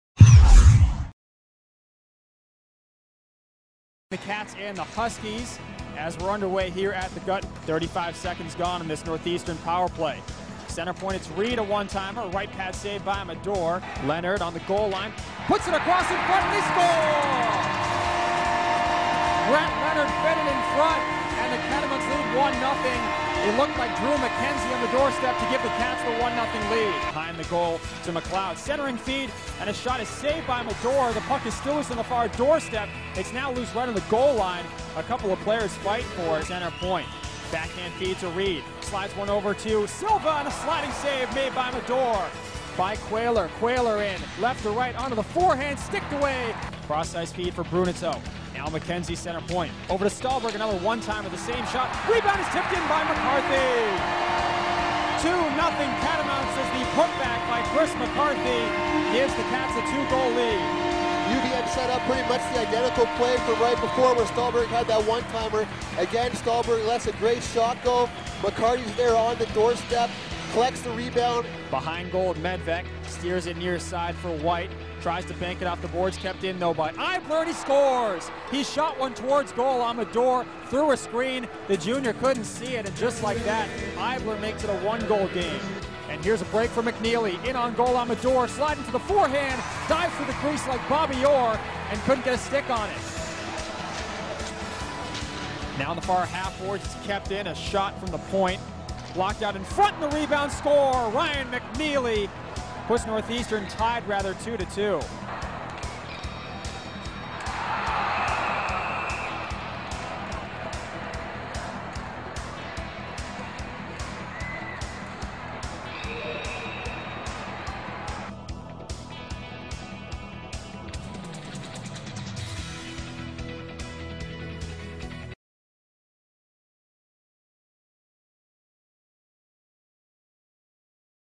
NORTHEASTERN AND VERMONT BATTLE TO 2-2 TIE
Senior forward Tyler McNeely scored 26 seconds into the third period as the Northeastern University Huskies skated to a 2-2 tie with the Vermont Catamounts on Friday, November 26, 2010 at Gutterson Field House.